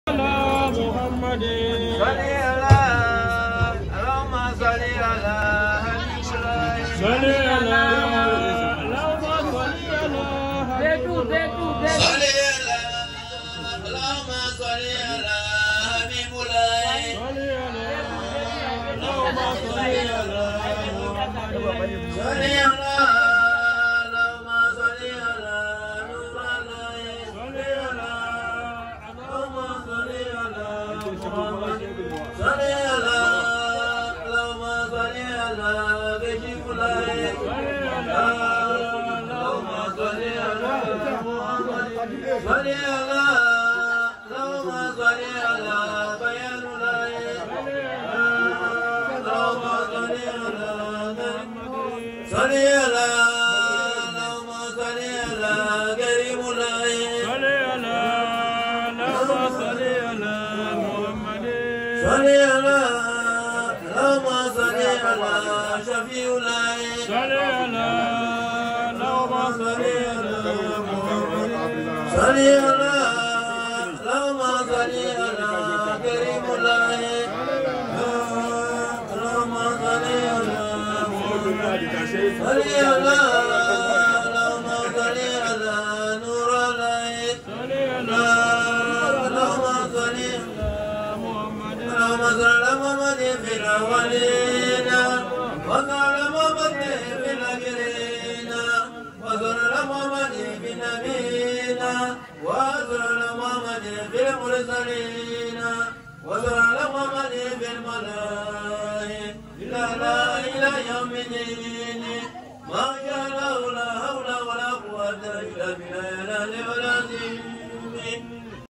Attachement de colas du couple Koné
Le mariage traditionnel de Coulibaly Myriam à odienné Côte ( attachement de colas)